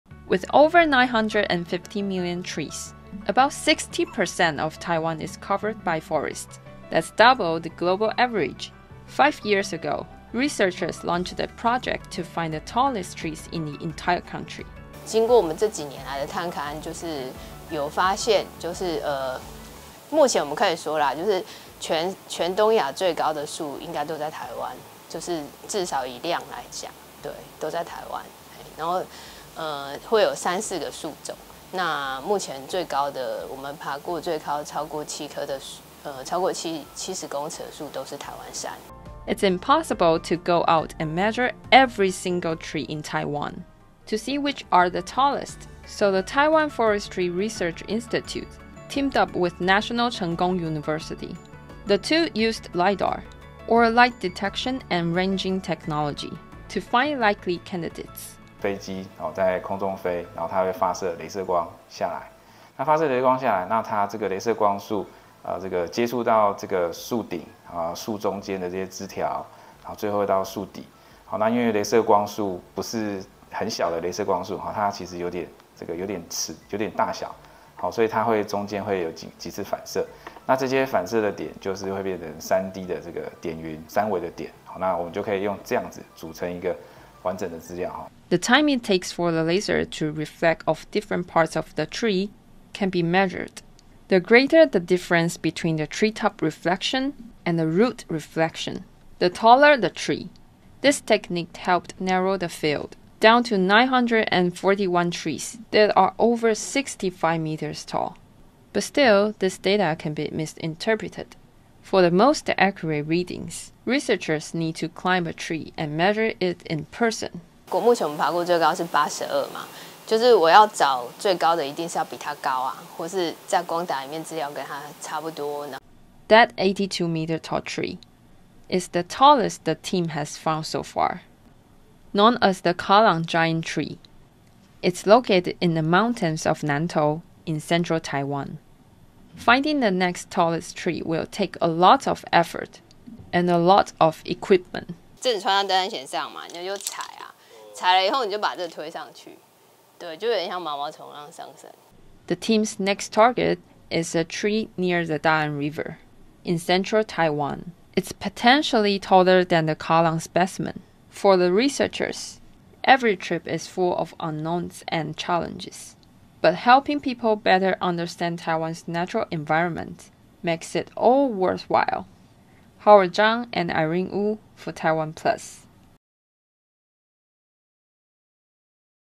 Searching for the Tallest Tree in Taiwan | TaiwanPlus News
📹Reporters: Howard Chang/Irene Wu

Taiwan Forestry Research Institute is using new technology to team up with National Cheng Kung University to find the tallest tree in Taiwan.